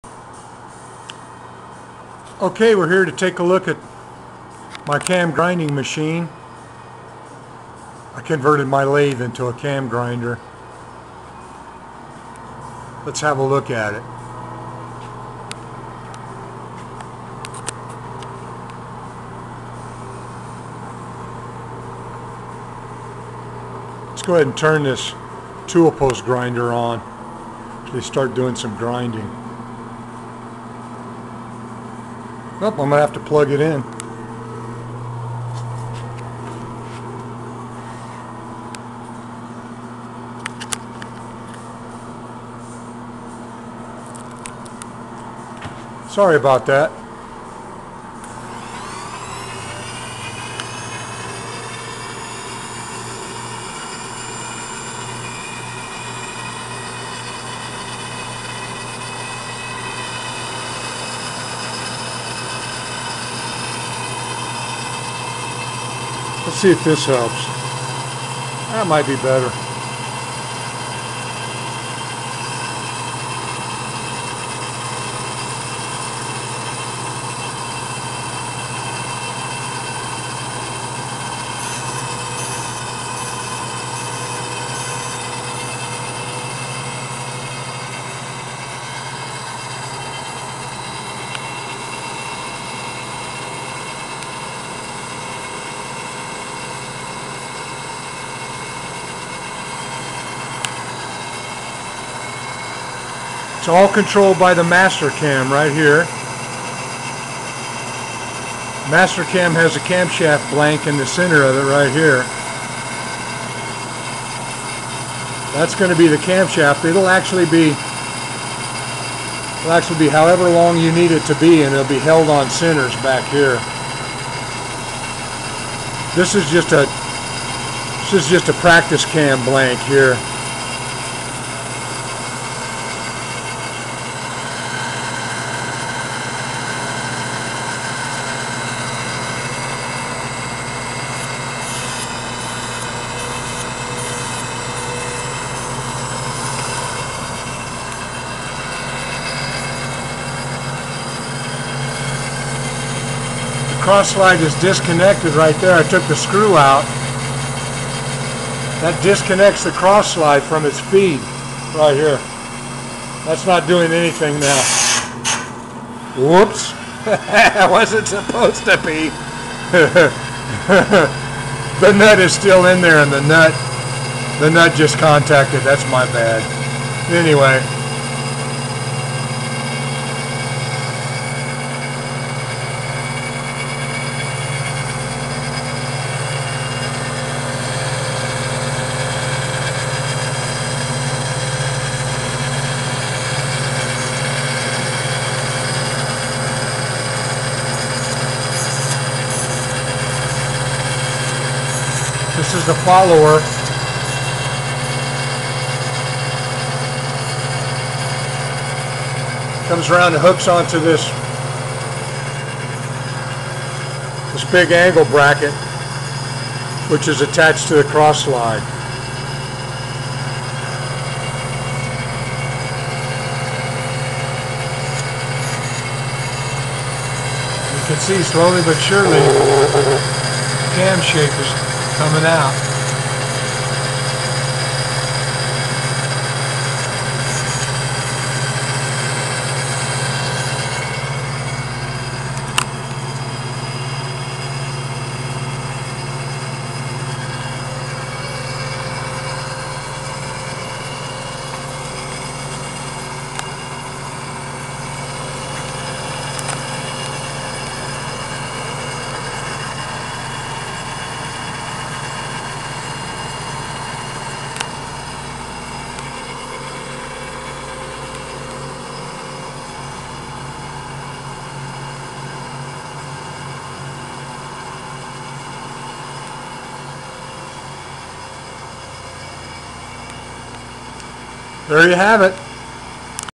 Wright Cam Grinder
I converted my lathe to grind scale model engine cams